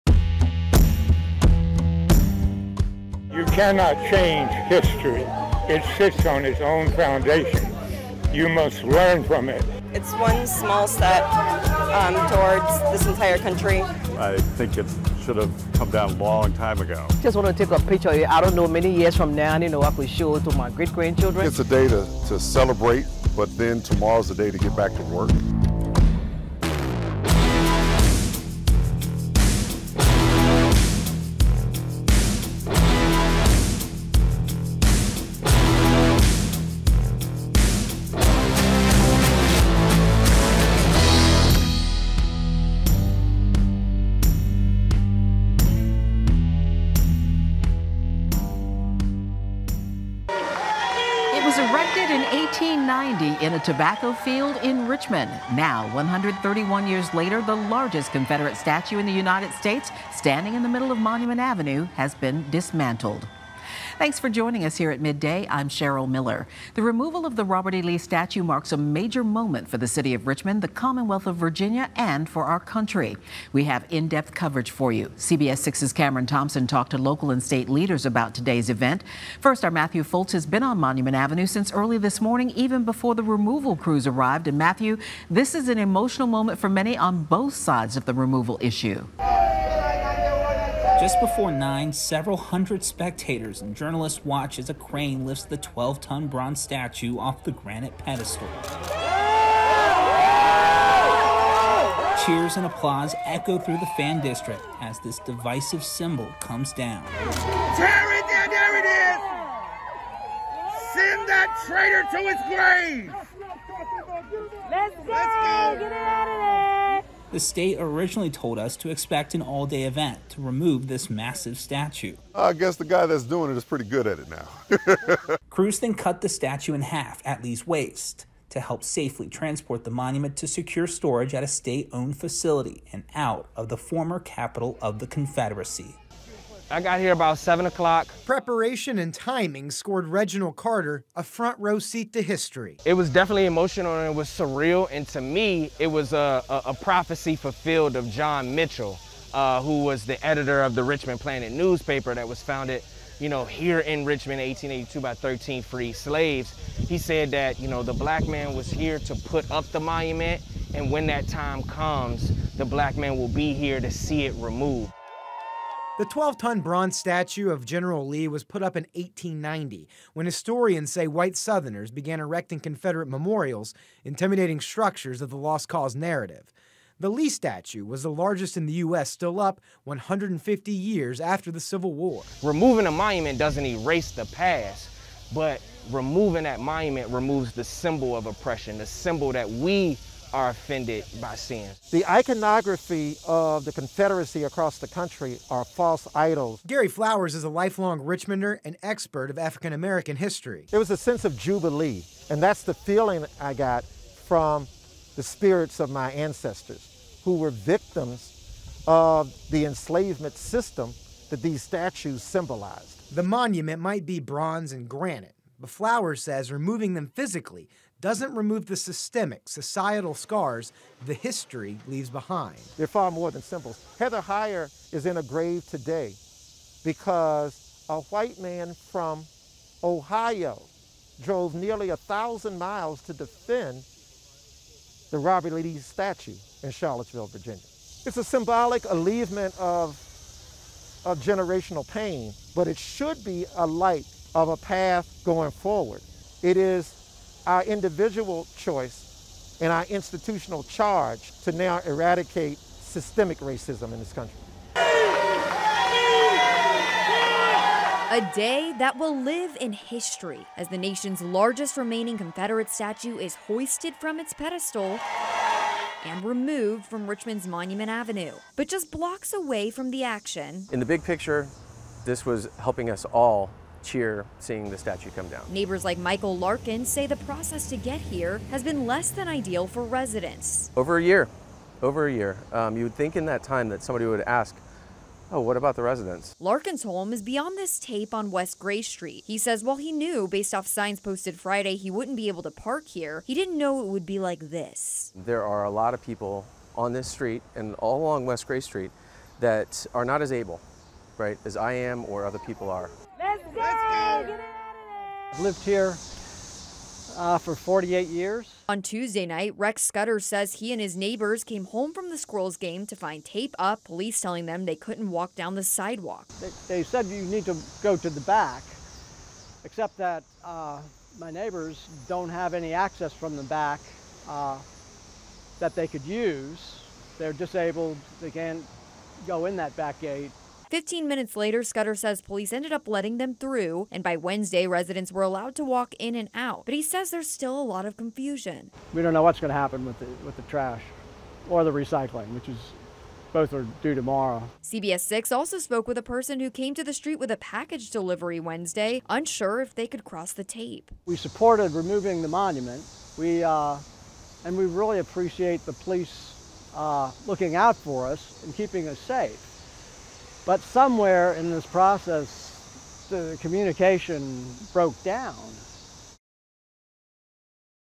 Robert E. Lee Statue Taken Down
AP Awards Entry, Best Team News Coverage

It had stood in the center of Richmond since 1890. Then, on June 4th, 2020, after a week of protests, the governor ordered that the statue of Robert E. Lee on Monument Avenue be taken down. That spurred a legal battle that lasted for more than a year. While Richmond's other confederate monuments were removed, the Lee statue remained in place. On September 2nd, 2021, the state's highest court ruled that the state had the authority to take it down. Less than a week later, before a crowd of hundreds, it happened.